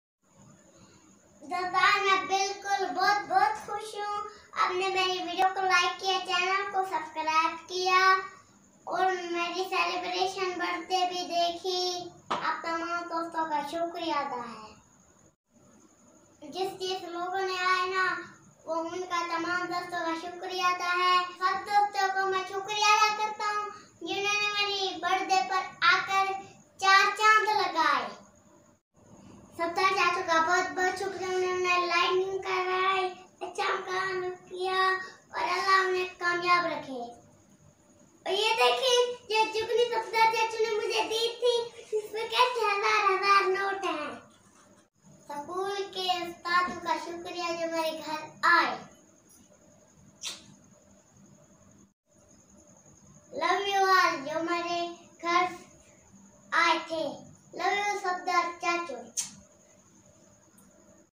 Thank You All | Anfal Gurmani Birthday Celebration | Birthday Decorations
Anfal Gurmani Birthday Celebrations & Decorations Thanks All friends for Coming Here